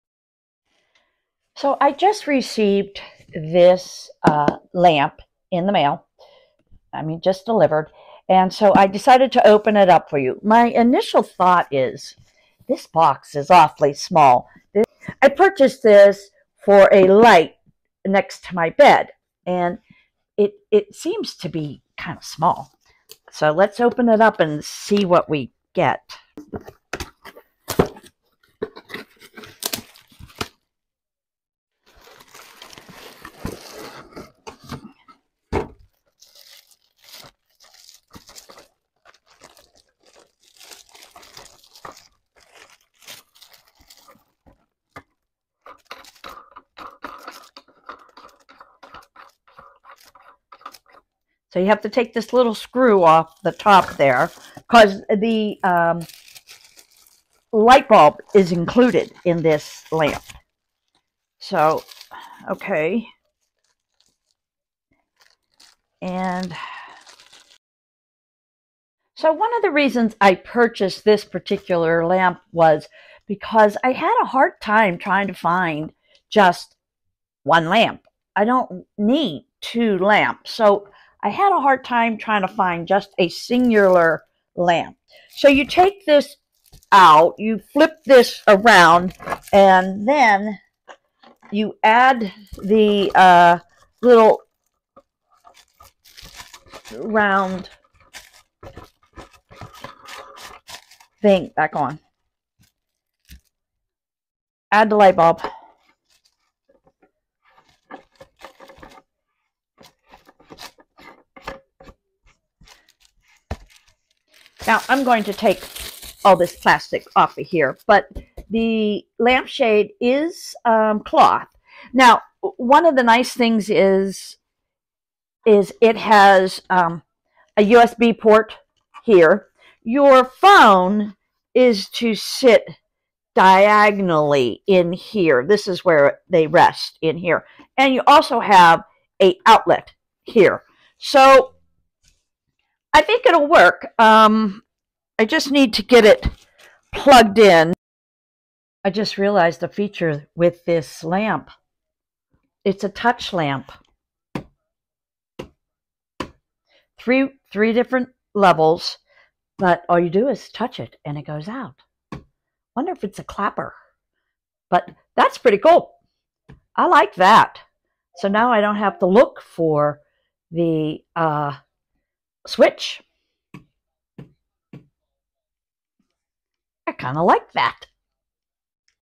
This Briever Table Lamp is the Perfect Bedside Lamp with USB Touch Control
Check out this cool lamp I picked up for my bedside. I didn't think I was going to like it but once I tried it out, I will never go back to a traditional style lamp.
This touch control bedside lamp provides 3-way adjustable brightness-Low, Medium, High. Easily tap on anywhere of the metal lamp base or the pole to choose the lighting setting as you need. Low mode is perfect for night light; Medium mode is ideal for daily use; High mode is great for reading or working. It can bring a warm and cozy touch to your house, making your home much more welcoming, and impress your guests.

 USB C Touch Control Table Lamp, 3-Way Dimmable Desk Lamp with & A and AC Outlet, Bedside Phone Stands for Bedroom, Living Room, Office, LED Bulb Included, Black

You can find all my additional links all in one place on my Linktr.ee.